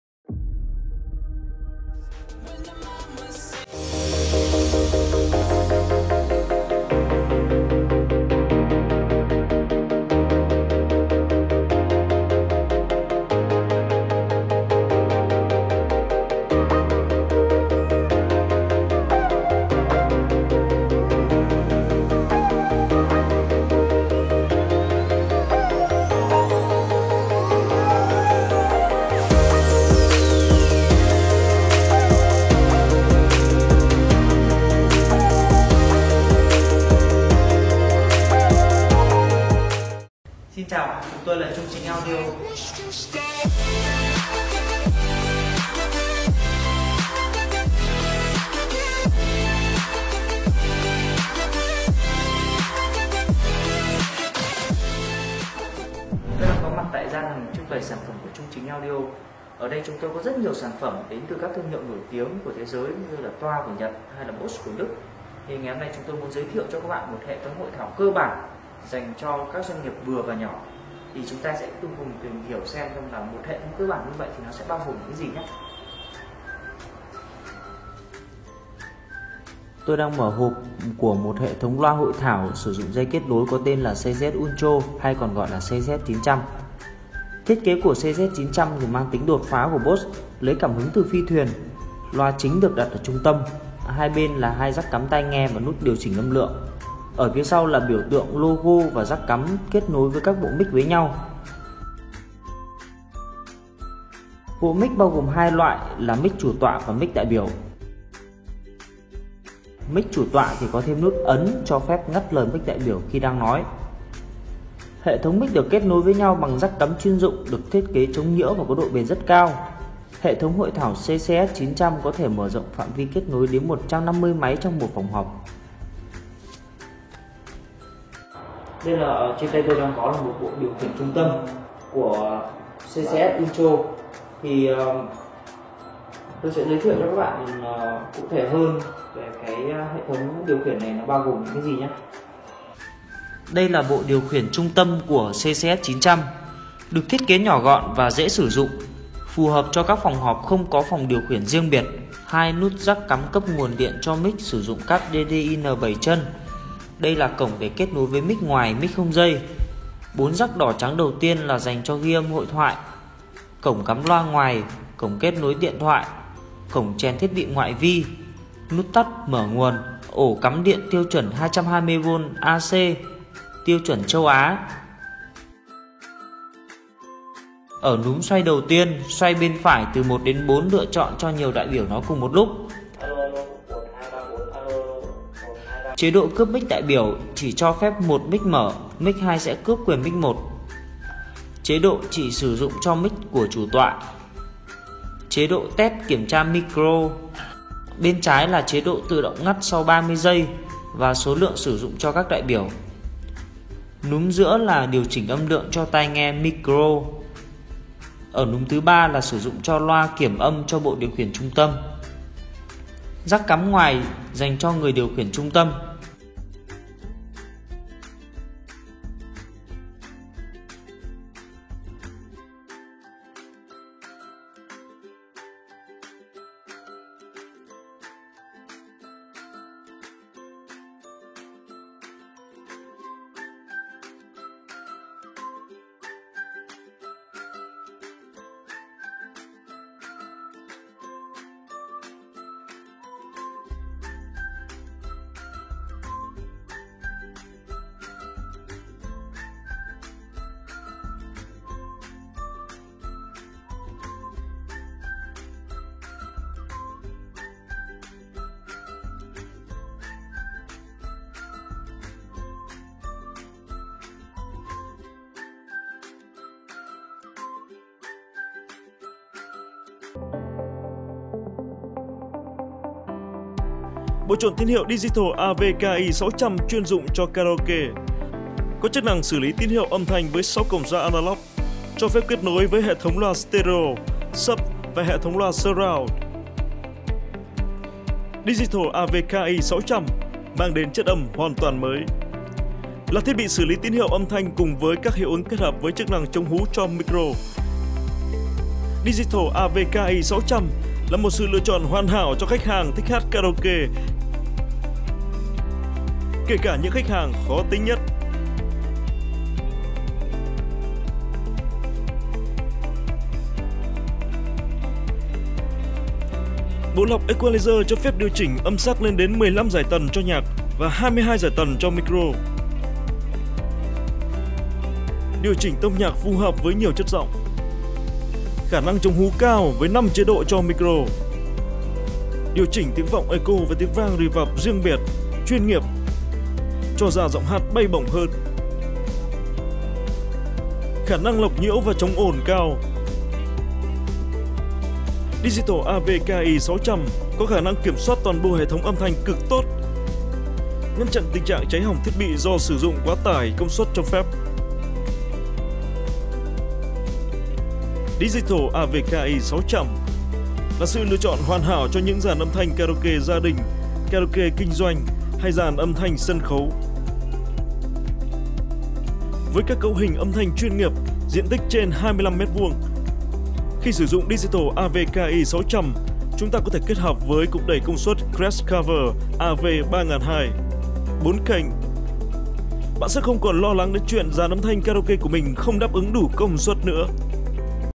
Lắp đặt BOSCH CCS1000D tại Chi Cục Thuế VĨNH YÊN - TRUNG CHINH AUDIO
Tư vấn lắp đặt hệ thống âm thanh phòng họp Chi cục thuế thành phố VĨNH YÊN, Vĩnh Phúc: Hệ thống hội thảo, âm thanh hội nghị trực tuyến BOSCH CCS 1000 cao cấp.
Địa chỉ: Số 1 Hà Huy Tập phường Tích sơn, Thành phố Vĩnh Yên, Vĩnh Phúc
Hệ thống âm thanh phòng họp BOSCH CCS-1000D
Thiết bị điều khiển trung tâm Bosch CCSD-CURD
Máy chủ tịch cần dài BOSCH CCSD-DL
Máy đại biểu cần dài BOSCH CCSD-DL
Cáp nối dài 10m Bosch LBB 4116/10
Loa hộp Bosch LB2-UC15-L1
Bộ khuếch đại Bosch Plena PLE‑1P240‑EU